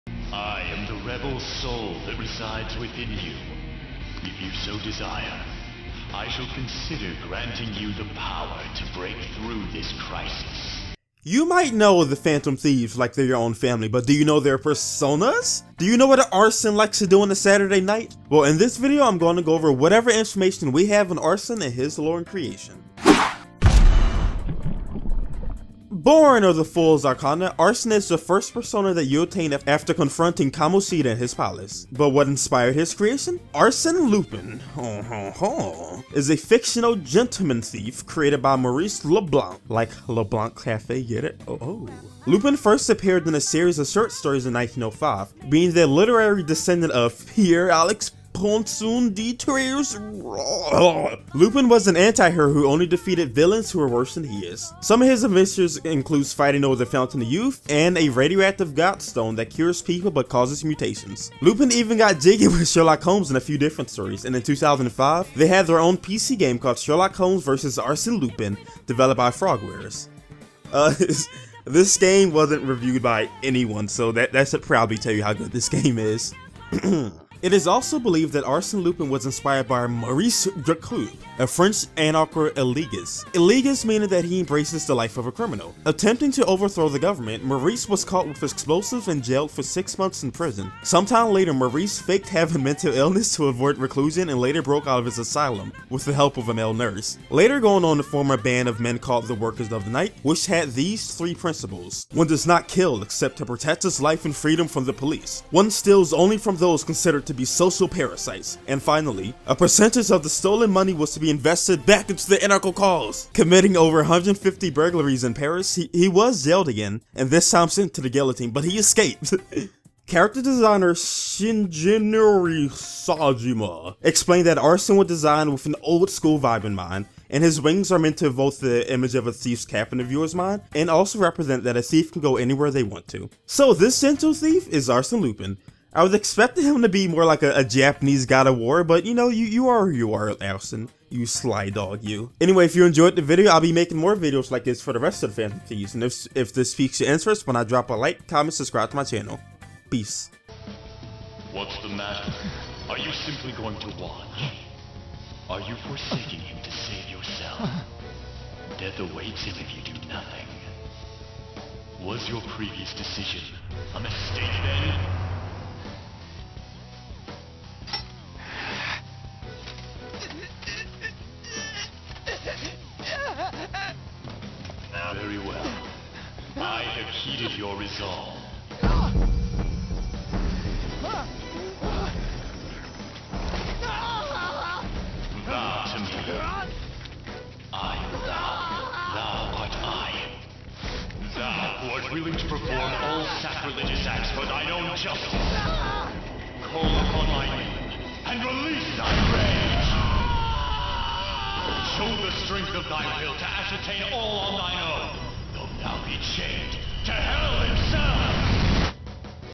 Persona 5 | The History Behind Arsene
Persona 5 is my favorite ps4 game and I wanted to do a video on my favorite group of friends. Not to many people know the lore and histroy of the persona's from the series. So in this video im going over the little information we have on arsene lupin, the model for the persona arsene. persona 5 lore.